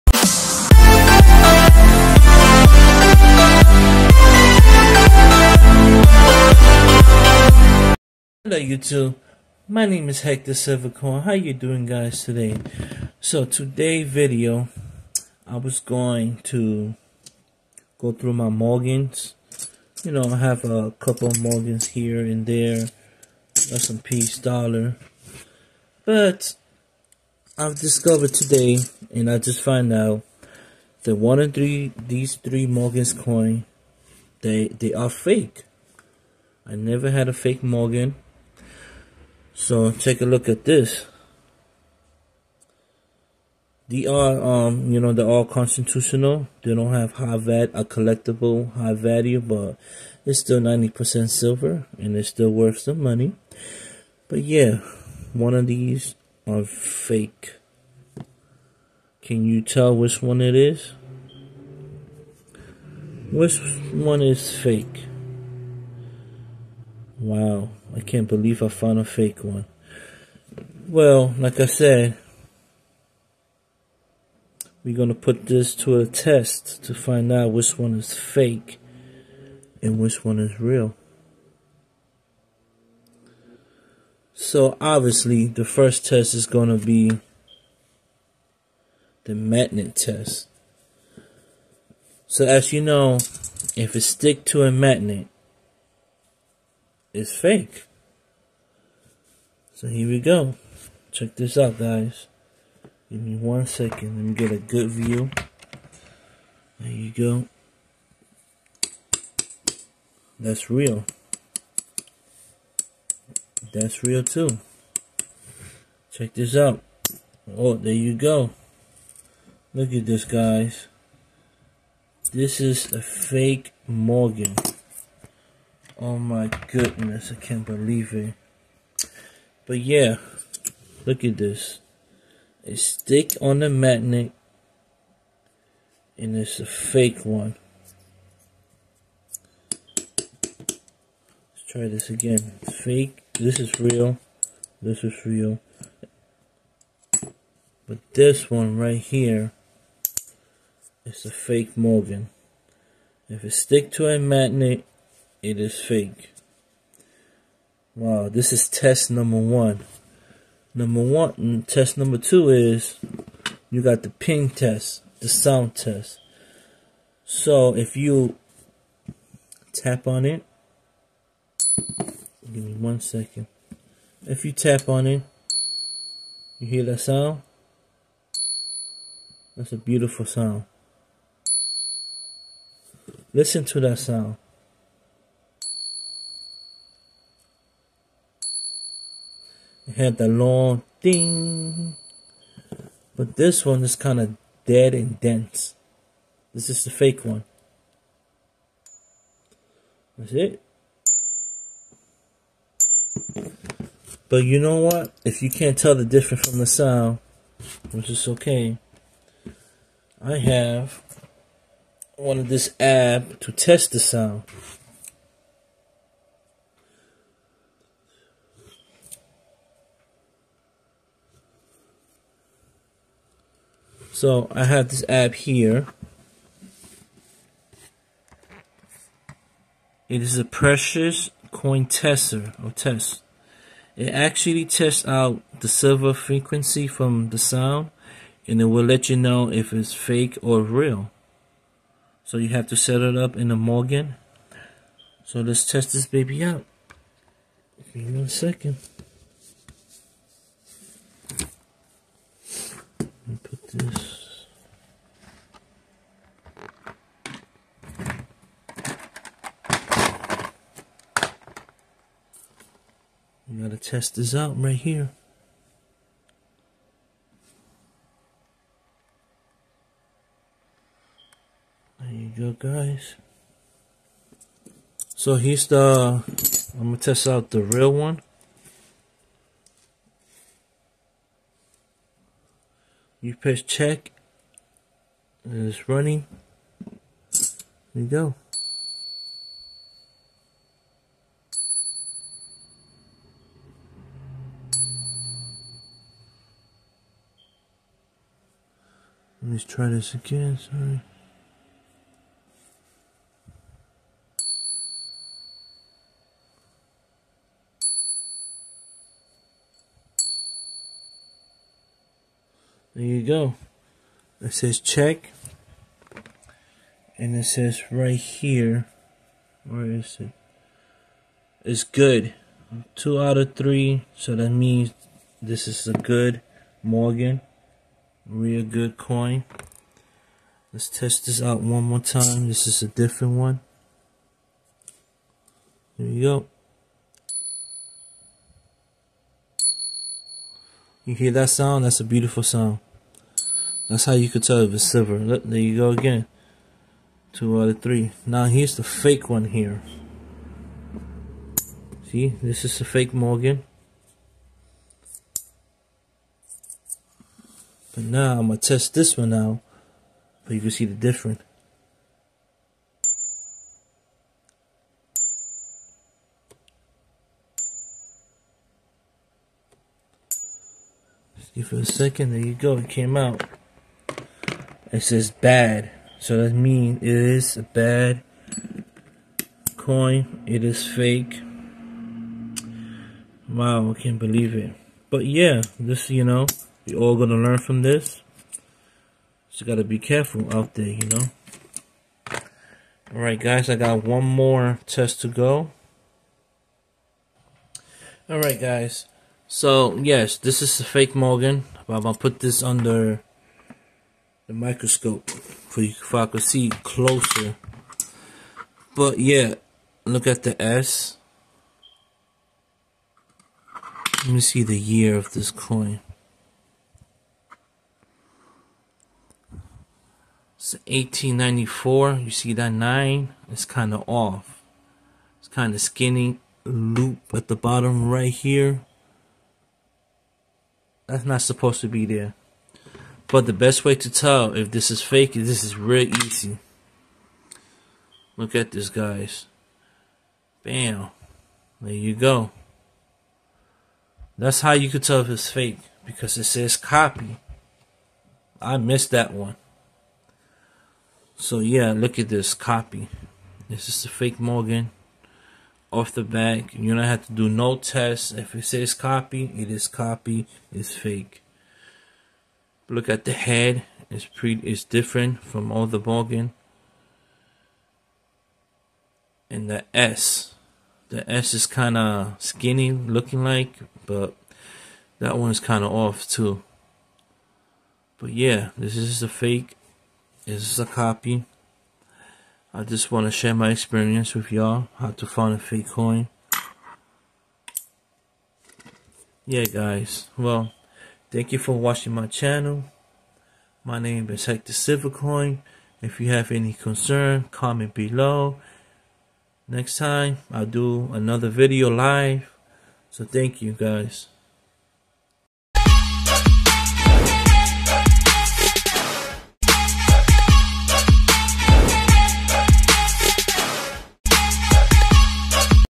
How to spot a fake Morgan?😁
In this video, I'm sharing how I found a fake Morgan dollar. This is a warning to all Morgan investors out there, be careful who you trust!

This fake Morgan dollar is a perfect example of why it's important to do your research before investing in any type of coin.
I found a fake Morgan in my collection and I put it to the test on a magnet..